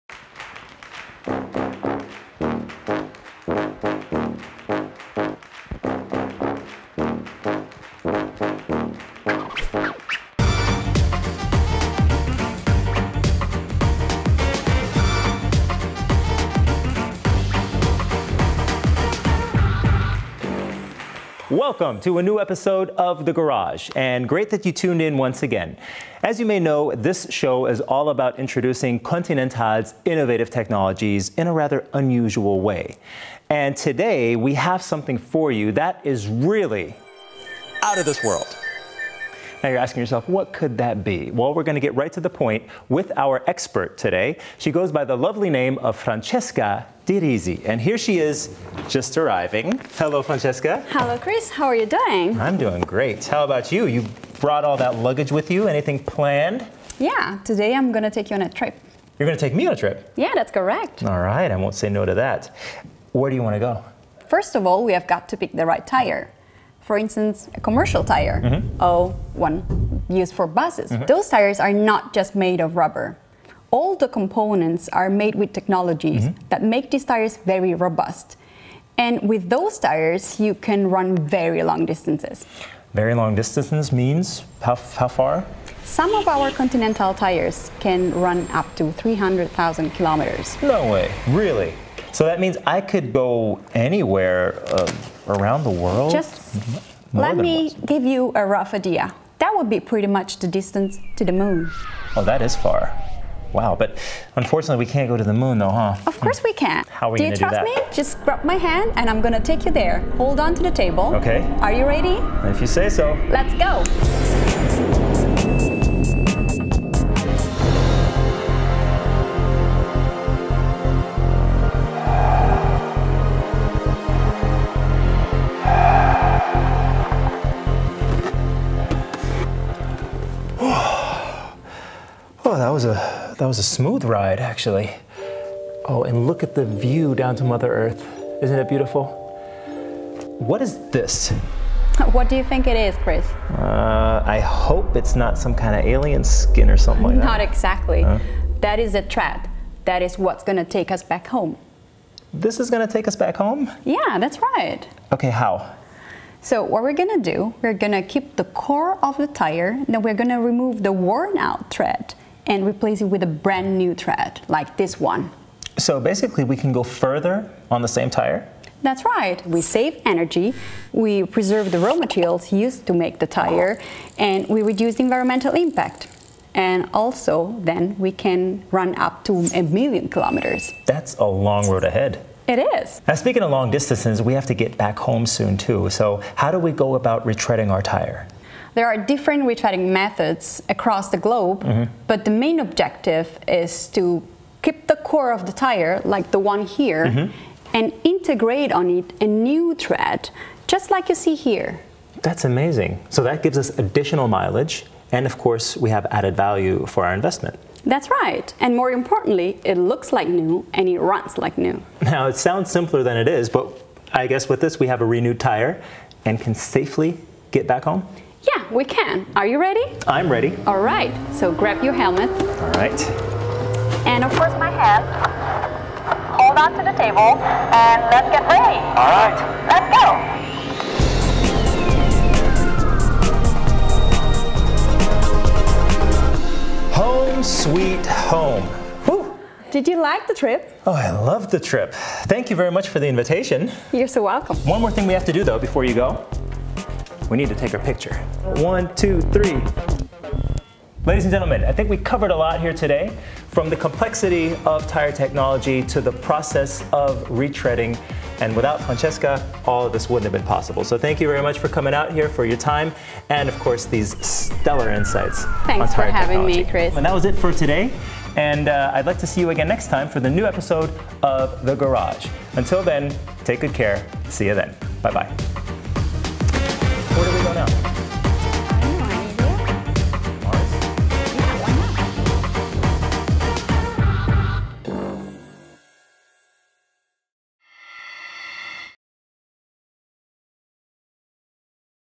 The Garage - Retreading Made Easy at Francesca's Space Station
The Garage is all about introducing innovative technologies in a rather unusual manner. This time we have something that is really out of this world! Join us as we take a trip to Francesca's Space Station, where retreading is made easy. We'll explain how some Continental tires can run up to a million kilometers thanks to robust technology and the high-tech process of retreading.